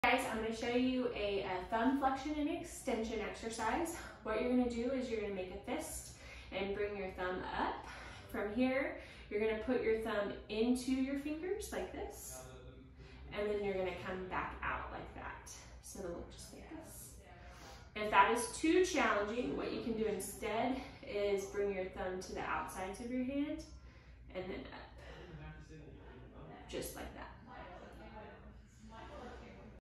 Thumb Extension and Flexion
Dr. Megan Comire of College Station Physical Therapy and Performance demonstrates a thumb extension and flexion.